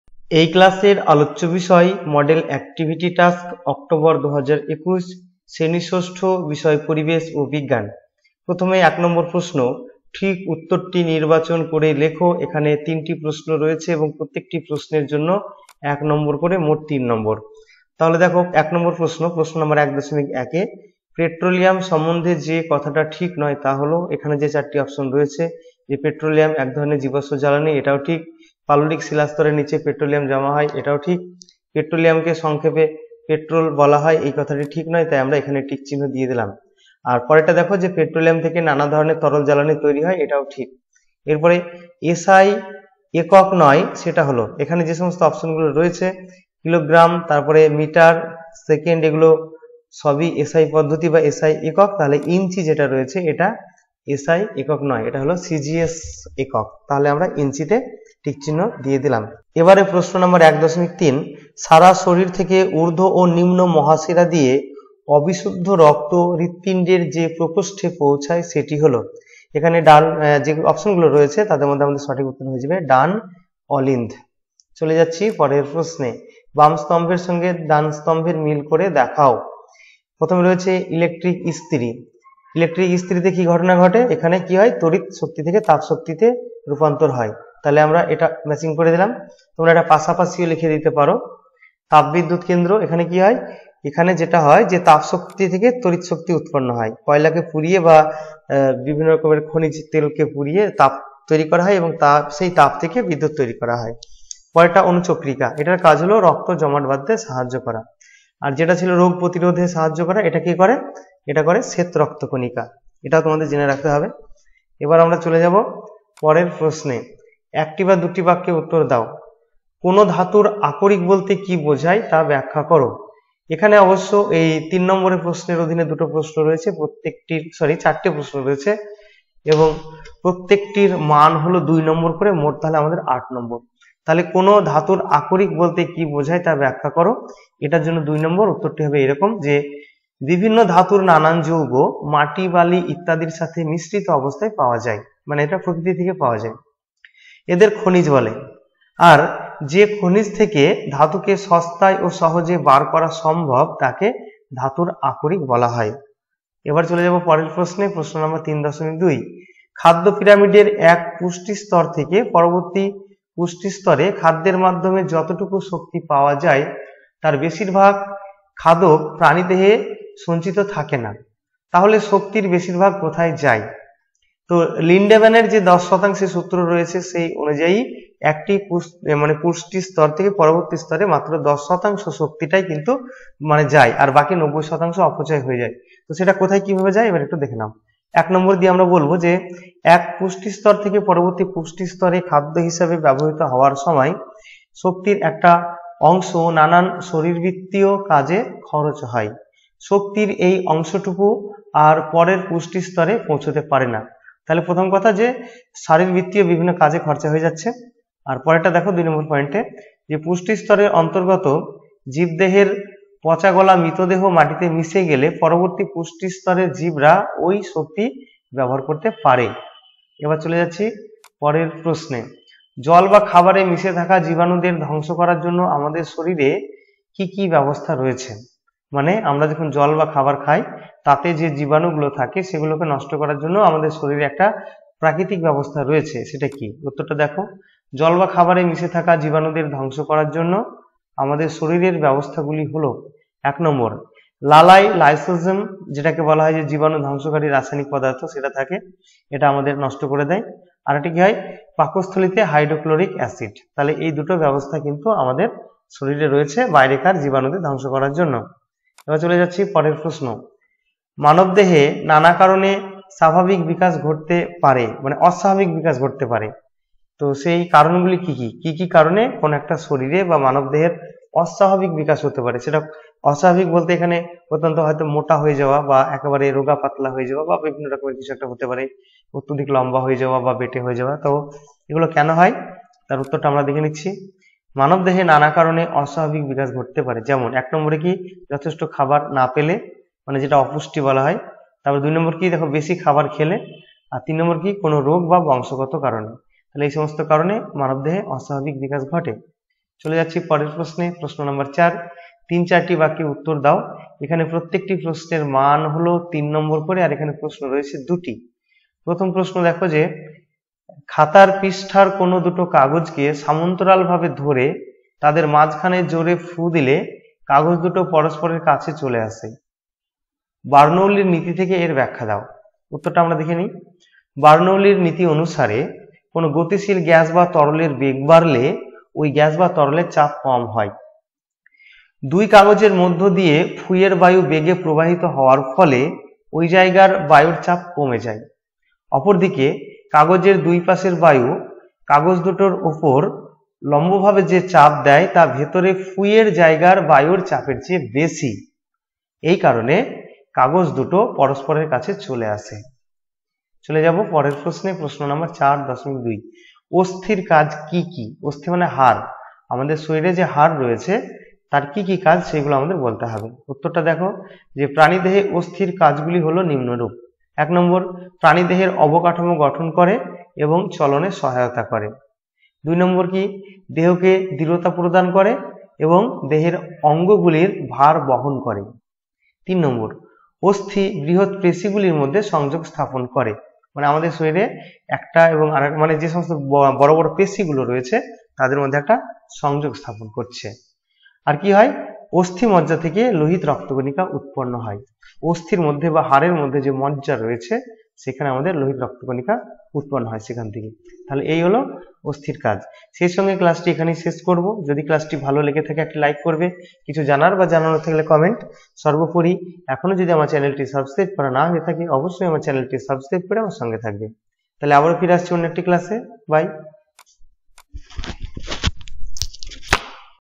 Class 6 Environment And Science Model Activity Task Part 7, October, 2021
The model activity task for class VI, Paribesh o bijyan/ Science, Part-7, given in the month of October 2021 (Task-IV) is being discussed here for the interest of the students. Thanking you,rkm,

7. Class- XI, Model Activity Task =
8. Class- XII Model Activity Task =